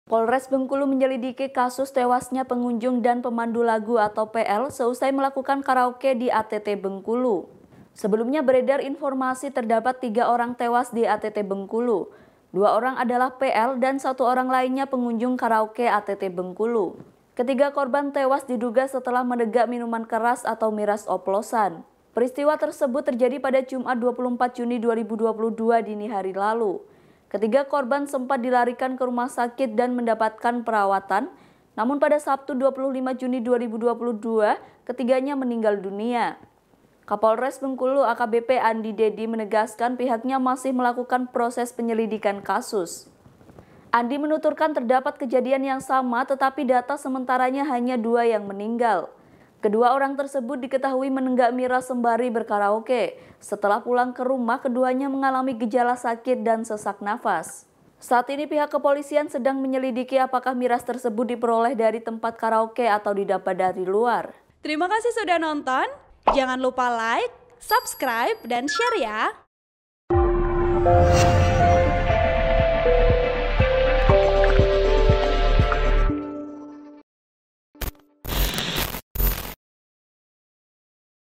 Polisi Selidiki Kasus Tewasnya Pengunjung dan Pemandu Lagu seusai Karaoke di ATT Bengkulu
TRIBUN-VIDEO.COM - Polres Bengkulu menyelidiki kasus tewasnya pengunjung dan pemandu lagu (PL) seusai karaoke di ATT Bengkulu.

Sebelumnya beredar informasi terdapat tiga orang tewas di ATT Bengkulu.

Dua orang adalah PL dan satu orang lainnya pengunjung karaoke ATT Bengkulu.

Ketiga korban tewas diduga setelah menenggak minuman keras atau miras oplosan.

Peristiwa tersebut terjadi pada Jumat (24/6/2022) dini hari lalu.

Ketiga korban sempat dilarikan ke rumah sakit dan mendapatkan perawatan.

Namun, pada Sabtu (25/6/2022) ketiganya meninggal dunia.

Kapolres Bengkulu AKBP Andi Dady menegaskan, pihaknya masih melakukan proses penyelidikan kasus.

Andi menuturkan terdapat kejadian yang sama tetapi data sementara hanya dua yang meninggal.

Kedua orang tersebut diketahui menenggak miras sembari berkaraoke.

Setelah pulang ke rumah, keduanya mengalami gejala sakit dan sesak nafas.

Saat ini pihak kepolisian sedang menyelidiki apakah miras tersebut diperoleh dari tempat karaoke atau didapat dari luar. (Tribun-Video.com/Tribunbengkulu.com)

Artikel ini telah tayang di Tribunbengkulu.com dengan judul BREAKING NEWS: Polisi Selidiki Kasus Tewasnya Pengunjung dan PL Usai Karaoke di ATT Bengkulu,
Penulis: Suryadi Jaya | Editor: M Arif Hidayat